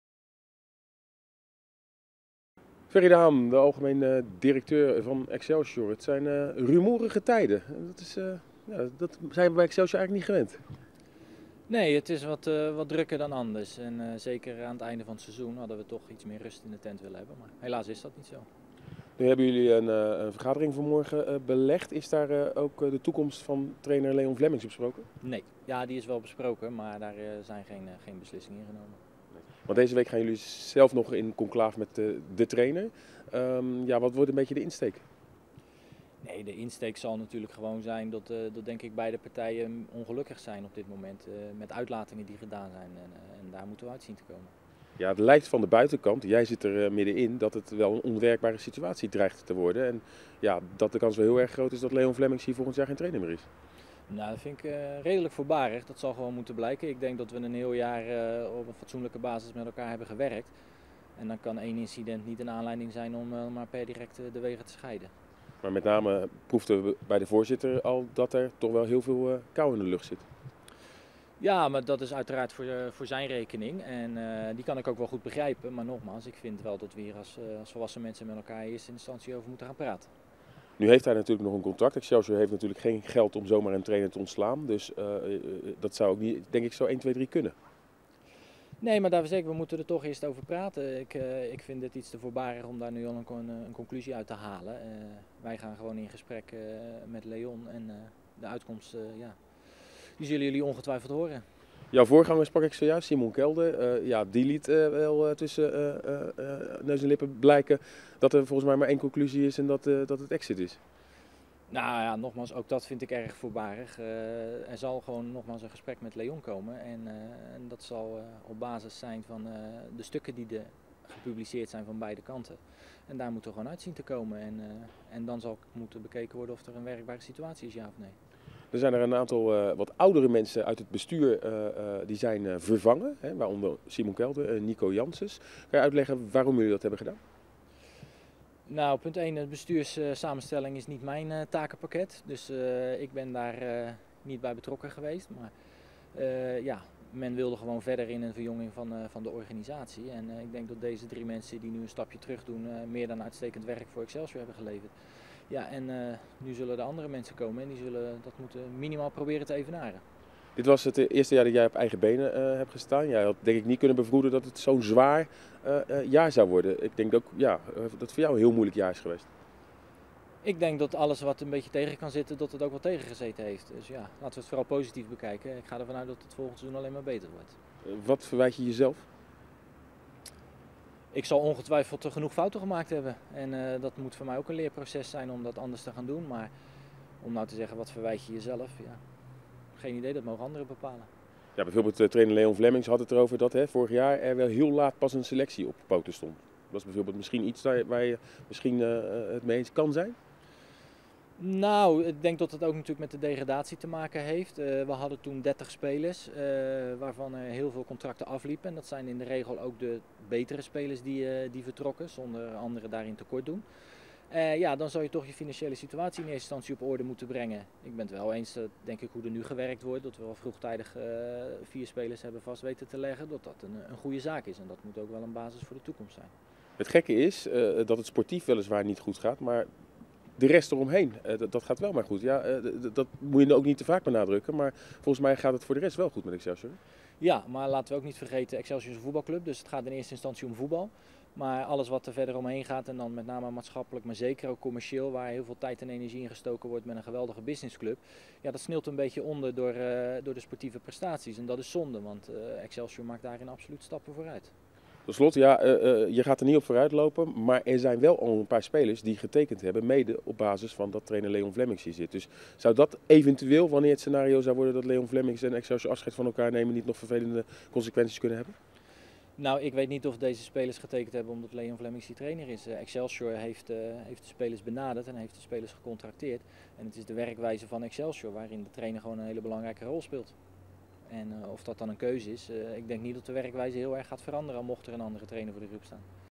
Ferry de Haan over de positie van trainer Leon Vlemmings
Simon Kelder noemt de manier waarop Excelsior-trainer Leon Vlemmings vorige week in de media heeft afgegeven op de club merkwaardig. 'Ik vind het absoluut niet correct dat hij Excelsior zo in diskrediet brengt.'